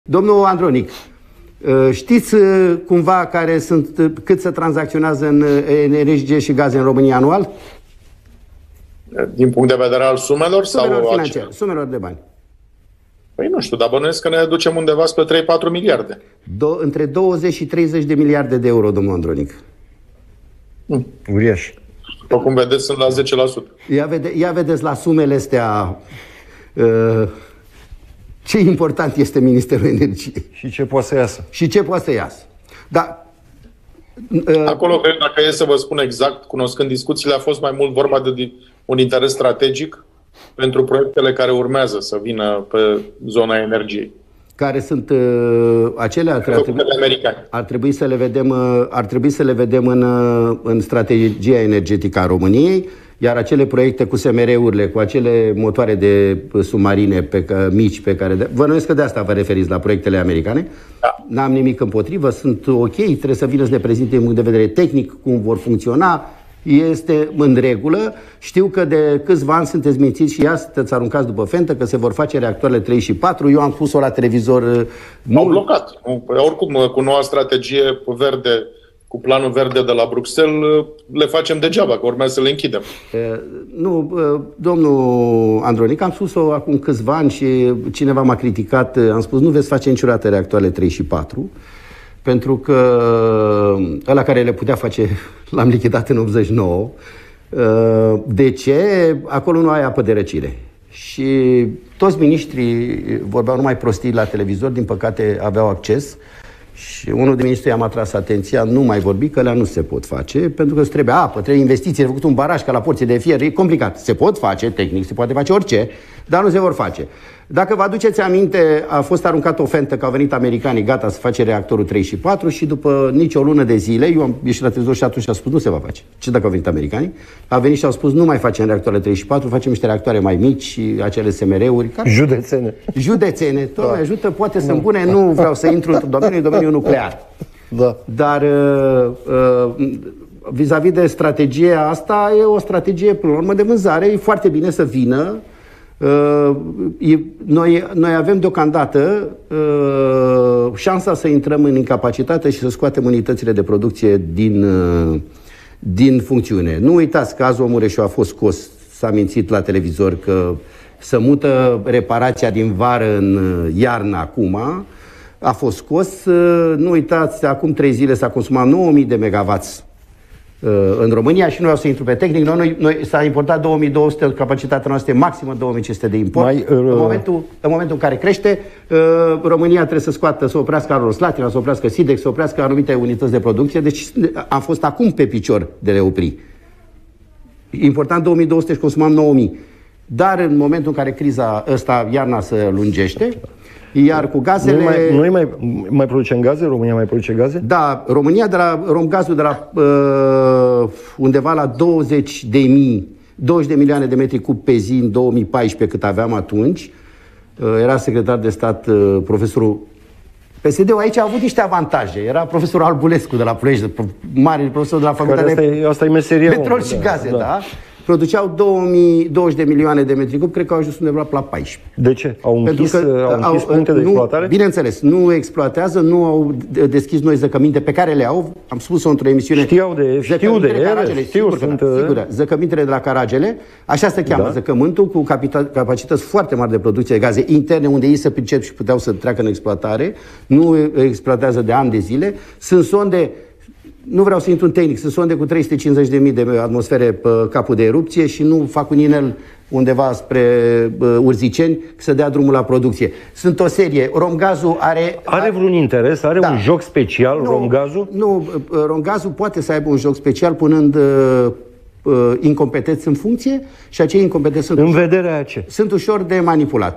Bombă pe piața de energie! Adio reactoarele 3 și 4? „E la fentă, suntem mințiți că se fac"
Speranțele de independență energetică a României au renăscut puternic odată cu anunțul autorităților din această vară că reactoarele 3 și 4 vor fi puse în funcțiune într-un viitor apropiat. Podcastul EVZ Play de vineri a discutat de ce ar putea fi o utopie acest proiect, mai ales că UE va avea un cuvânt greu de spus.

Tranzacțiile pe piața de energie și gaze se ridică la o sumă imensă în România, cuprinsă între 20-30 miliarde de euro, a explicat Dian Popescu, fost senator liberal, specialist în energie. Invitatul moderatorilor Dan Andronic și Mirel Curea a dezvăluit informații din culise legate de șansele de punere în funcțiune a reactoarelor 3 și 4 de la Cernavodă.

Dian Popescu: Adio reactoarele 3 și 4! Ăla care le putea face l-am împușcat

„Vedeți la sumele astea ce important e Ministerul Energiei. Proiectele cu americanii ar trebui să le vedem în strategia energetică a României. Acele proiecte cu motoarele de submarine mici (n.r. - semnate cu SUA în vara lui 2021) sunt OK. De câțiva ani suntem mințiți. Cineva mi-a spus că nu vom putea face niciodată reactoarele 3 și 4, ăla care le putea face l-am lichidat în 89. De ce? Acolo nu ai apă de răcire, trebuie investiții, trebuie un baraj ca la Porțile de Fier. A fost aruncată o fentă că au venit americanii. Am ieșit la televizor și am spus că nu se va face. Americanii au venit și au spus că facem acele reactoare mai mici. Nu uitați că Azomureș a fost scos din funcțiune. Romgaz producea 20 milioane de metri cubi de gaz pe zi, acum a ajuns pe la 14 milioane. Nu exploatează, nu au deschis noi zăcăminte pe care le au. Dar dacă iarna se lungește?", a explicat Dian Popescu.

Dian Popescu: E o încrengătură între companii. Suntem în al 12-lea ceas

În opinia invitatului, statul ar trebui să fie mult mai ferm în relație cu companiile la care deține acțiuni. „Să mai salvăm ce se mai poate salva. Romgaz nu a mai forat. Au înlăturat specialiștii și au adus toți impostorii. Au fost puși numai pe criterii politice. Dacă prăbușim aceste companii, cine răspunde? Când statul devine partener în marile companii, acestea să aibă obligații", a punctat fostul senator Dian Popescu.